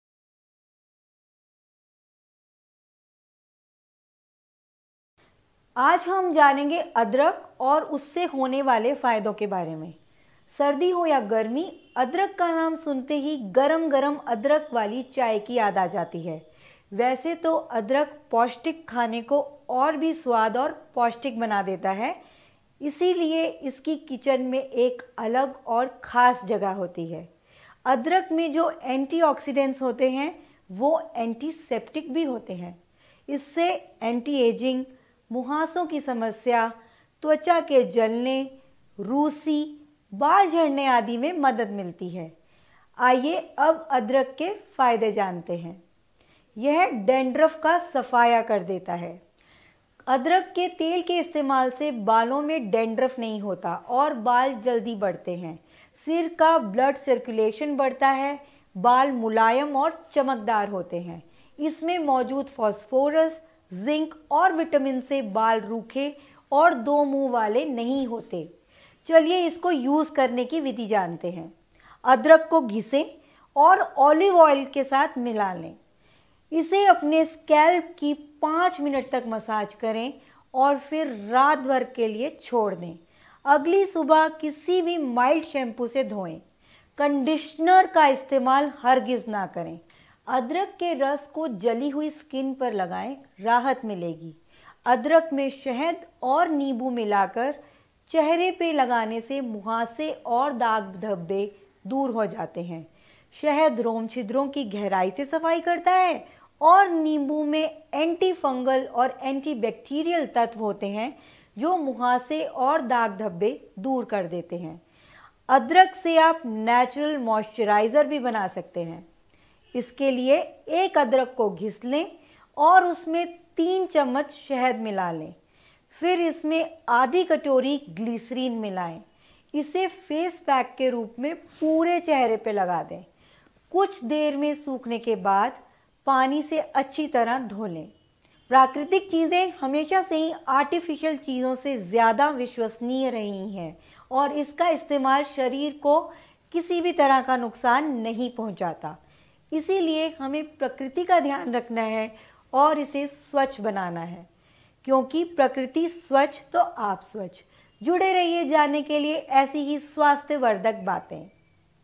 क्या आपको अदरक के इन फायदों के बारे में पता था ? । Health Benefits of Ginger | Sanjeevni Health
अदरक के फायदे, Health Benefits of Ginger, अदरक के सेवन के फायदे, Health Benefits Of Ginger Juice, Adrak Ke Fayde, अदरक के जूस के फायदे

Ginger is Around those healthiest (and The greater part delicious) spices on the planet. It will be stacked for supplements What's more bioactive exacerbates that need capable reductions to your physique and mind. Ginger camwood be utilized fresh, dried, powdered, or Similarly as an oil alternately juice, Furthermore may be Frequently included on transformed nourishment. What's more cosmetician. It may be a basic element to recipes. Those exceptional aroma What's more flavor of ginger turned starting with its characteristic oils, The greater part essential of which will be gingerol.

Health Benefits
•       Ginger is a Powerful Cancer Fighter
•       Anti-Inflammatory Properties Help with Many Conditions
•       Cough
•       Sore Throat
•       Headaches/Migraine